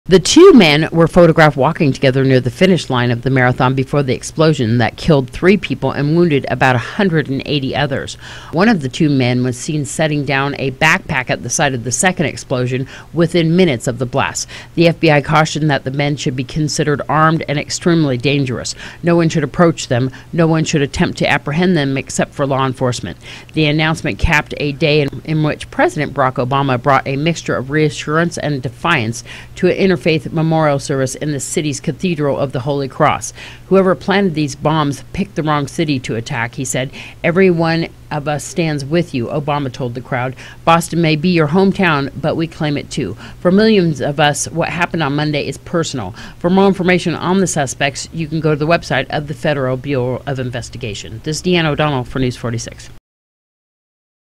04/18/2013 Boston Bombing Suspects Photos
The FBI released photos and video of two men it called suspects in the deadly bombings at the Boston Marathon and pleaded for public help in identifying them.